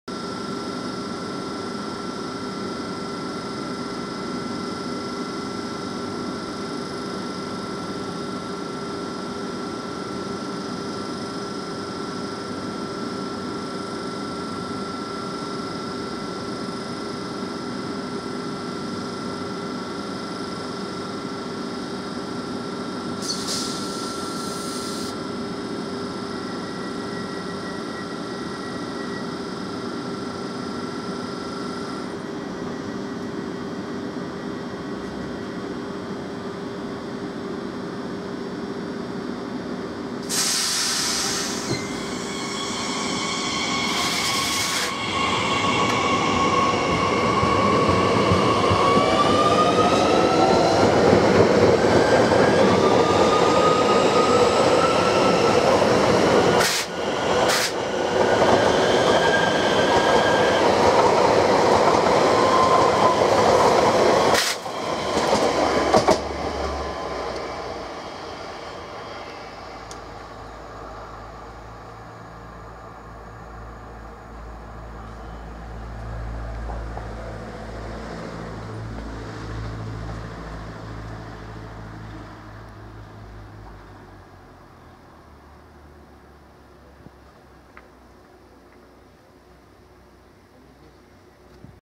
Z50000 NAT : Départ de la gare de Gros Noyer Saint Prix sur la ligne H du Transilien
Départ d'une rame Z50000 NAT de la gare de Gros Noyer Saint Prix sur la ligne H du Transilien .
Transilien ligne H .
Réseau SNCF .